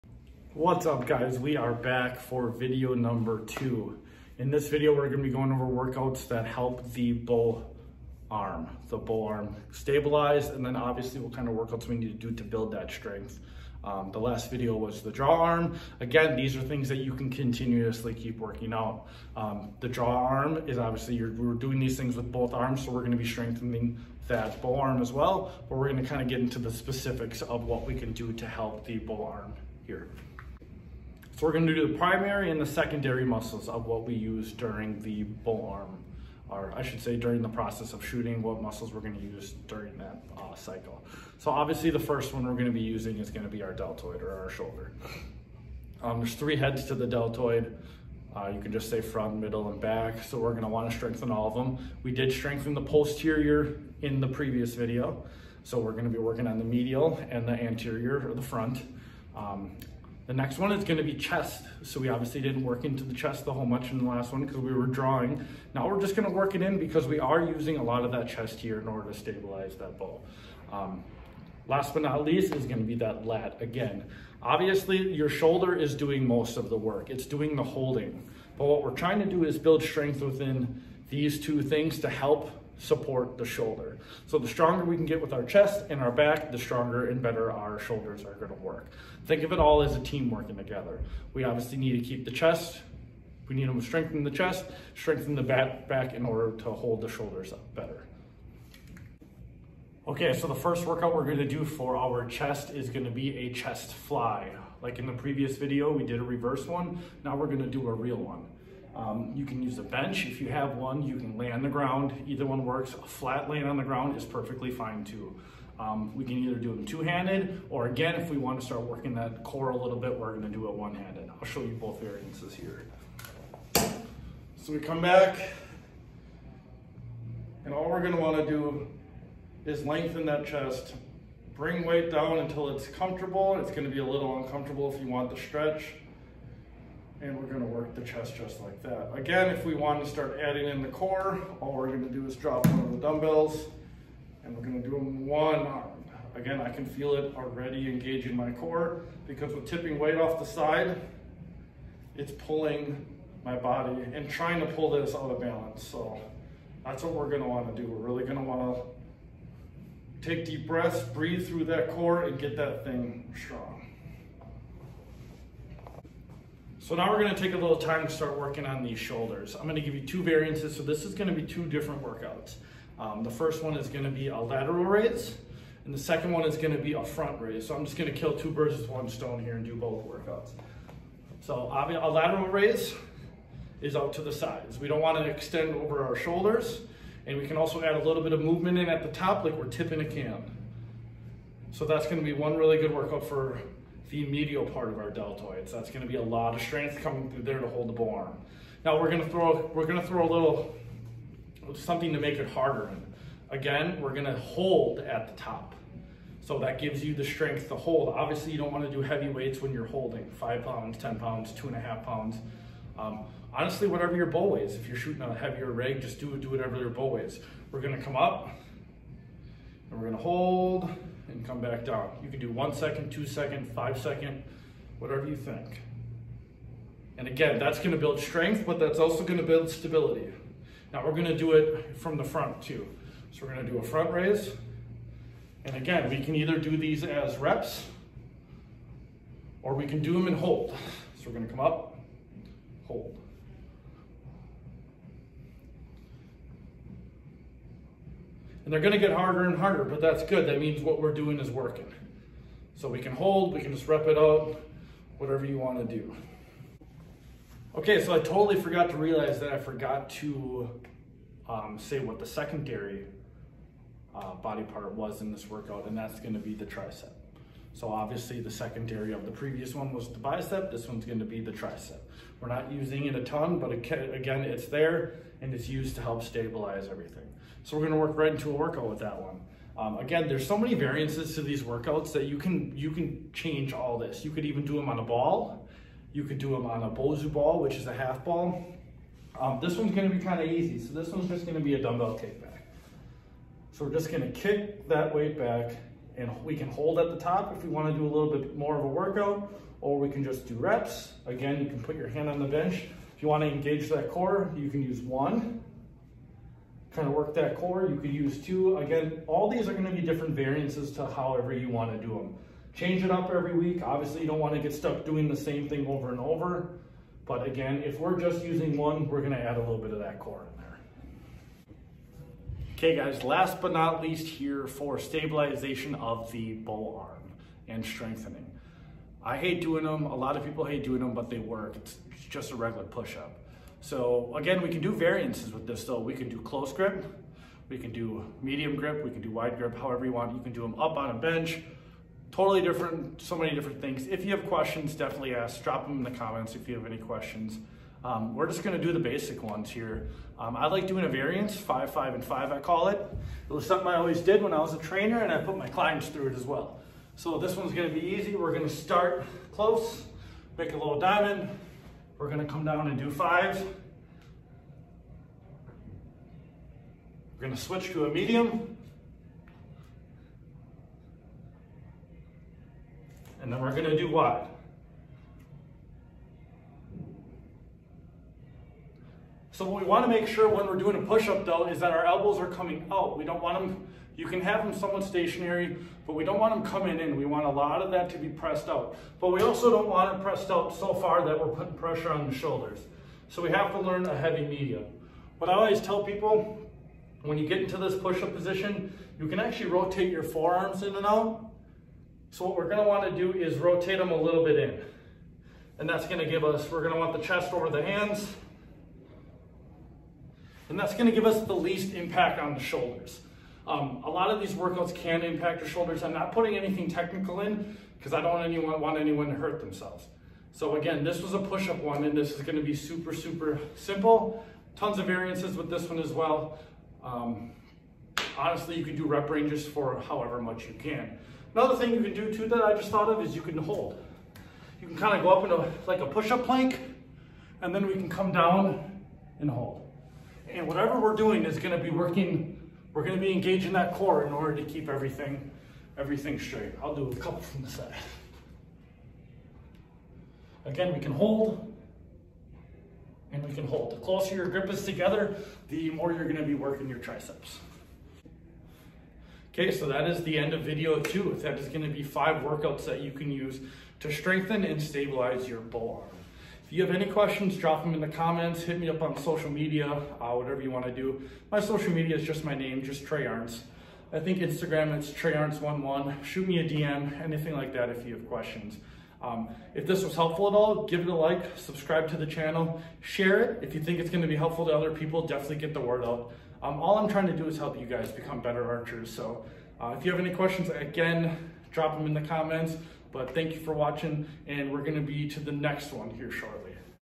Archery Workouts - BOW ARM
Video #2 of the Archery Workouts series. This one features workouts to help improve strength and stability within the bow arm.